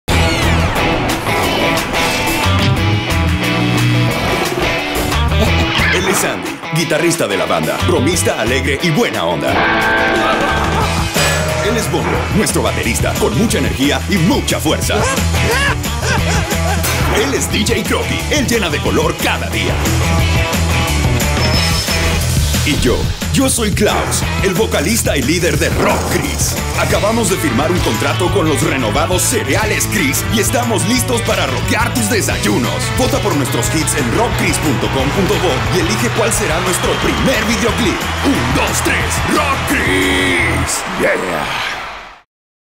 Spot RocKris - Cereales Kris
La empresa Kris, a través de su agencia Rock And Roll, nos encargó darle vida a los 4 personajes de su línea de cereales en un comercial 100% 3D. Estamos muy contentos con el resultado de una complejidad nunca antes alcanzada en Bolivia!

Producción: Mamba Studio
Dirección: Daniel Terrazas
Dirección de Arte: Pablo Miño
Modelado de Personajes: Rodrigo Fernandez
Supervisor de Animación: Alejandro Villavicencio V.
Modelado: Ariel Terrazas, Daniel Terrazas
Iluminación y render: Daniel Terrazas
Composición: Alejandro Villavicencio, Daniel Terrazas
Audio: Escapa Estudio

Por el cliente: Ivan Dorado
Dirección creativa general: Favio Encinas
Dirección Creativa: Paula Suárez
Cuentas: Willans Teran